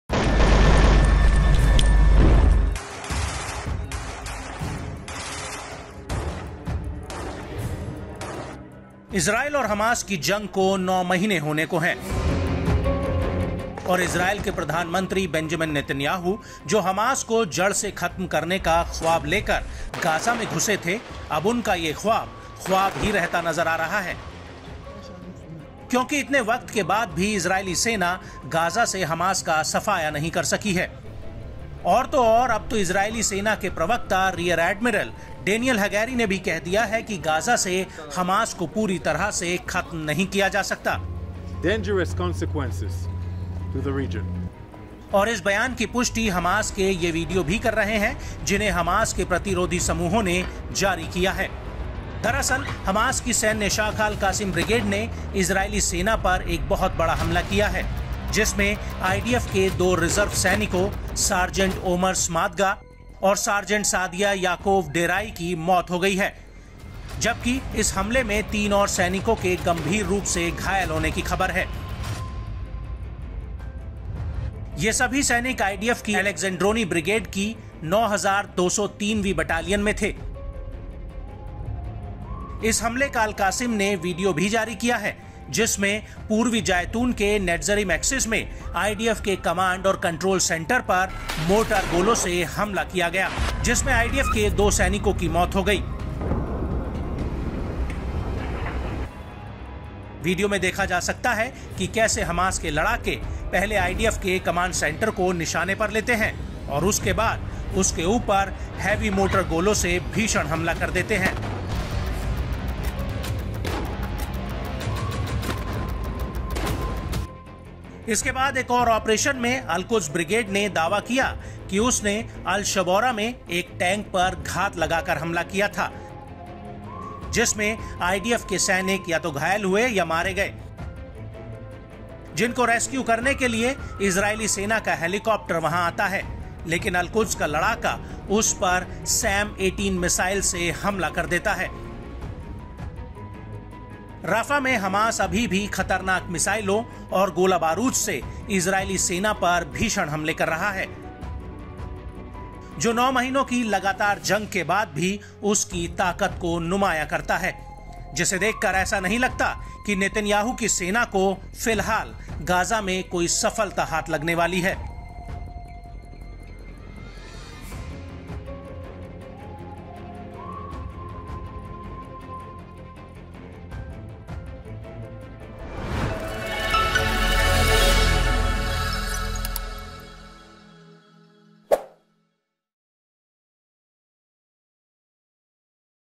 Israel Hamas War : हमास का इज़राइली कमांड सेंटर पर भीषण अटैक ! | Al Qassam| Netanyahu | N18G
It is about to be 9 months since the war between Israel and Hamas and Israeli Prime Minister Benjamin Netanyahu, who had entered Gaza with the dream of rooting out Hamas, now seems to have remained a dream because even after so much time, the Israeli army Could not eliminate Hamas from Gaza. What's more, now Israeli Army spokesperson Rear Admiral Daniel Hagari has also said that Hamas cannot be completely eliminated from Gaza.

Israel और Hamas की जंग को 9 महीने होने को हैं और इज़रायल के प्रधानमंत्री Benjamin Netanyahu जो हमास को जड़ से ख़त्म करने का ख्वाब लेकर ग़ज़ा में घुसे थे अब उनका ये ख्वाब, ख्वाब ही रहता नज़र आ रहा है क्योंकि इतने वक़्त के बाद भी इज़रायली सेना ग़ज़ा से हमास का सफाया नहीं कर सकी है। और तो और अब तो इज़रायली सेना के प्रवक्ता रियर एडमिरल डेनियल हगारी ने भी कह दिया है कि ग़ज़ा से हमास को पूरी तरह से ख़त्म नहीं किया जा सकता.